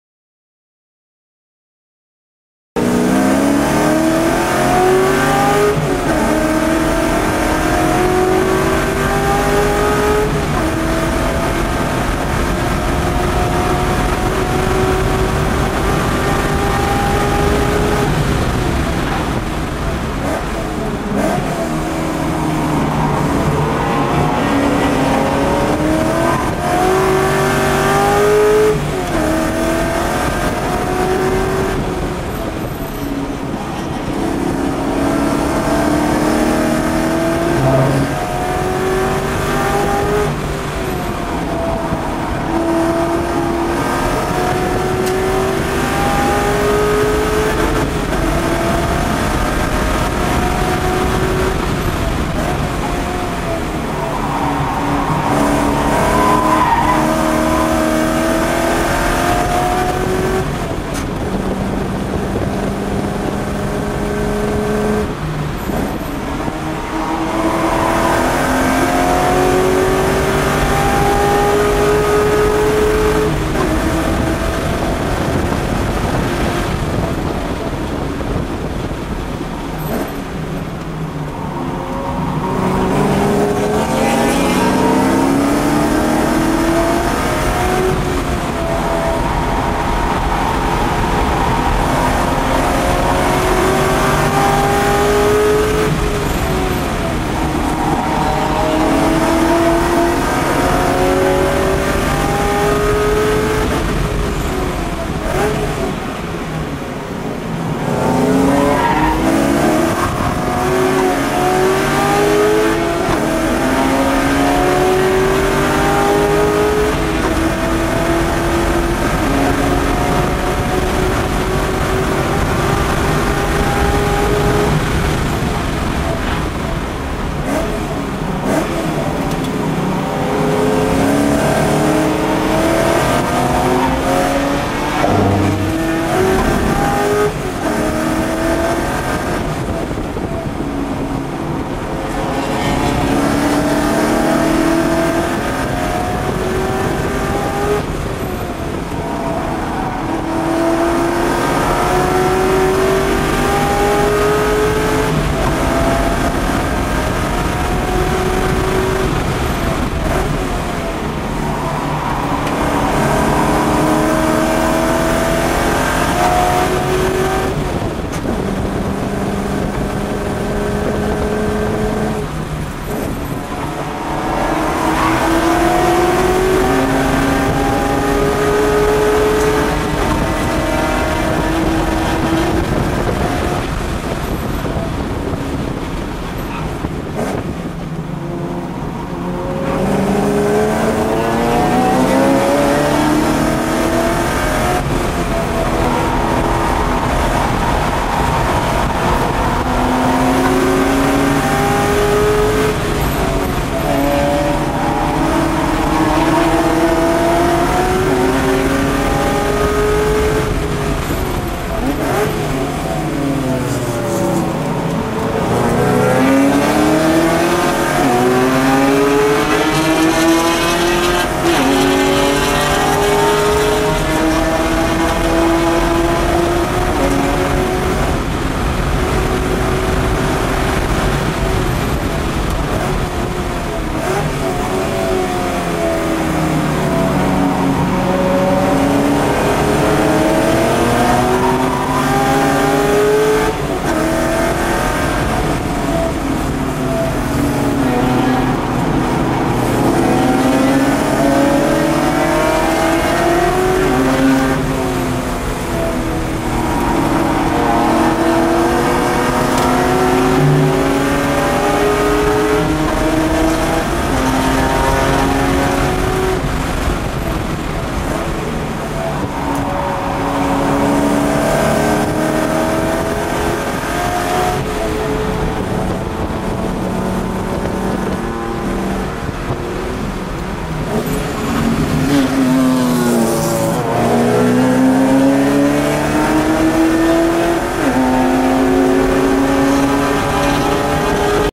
Porsche 928 @ Laguna Seca
Open track day w/ modified 928S4 at Laguna Seca w/ Trackmasters.  Pirelli P-Zero Corsa Asymmetricos.  All downshifts done w/ heel & toe and double clutch technique.